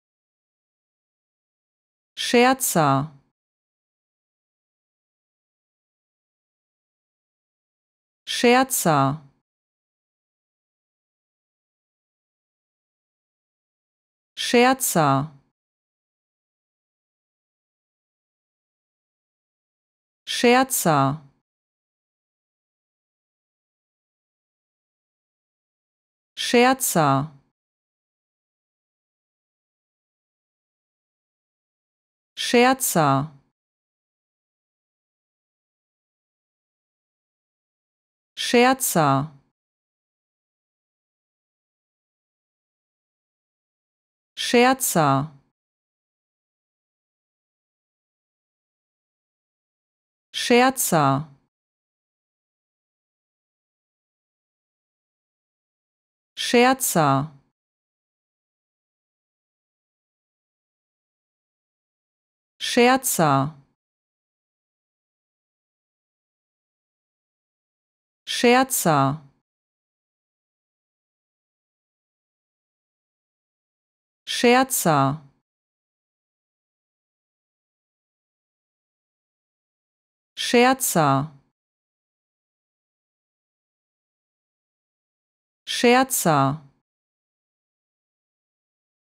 How to Pronounce ''Scherzer'' Correctly in German
Learn how to say ''Scherzer'' in German with this short pronunciation tutorial.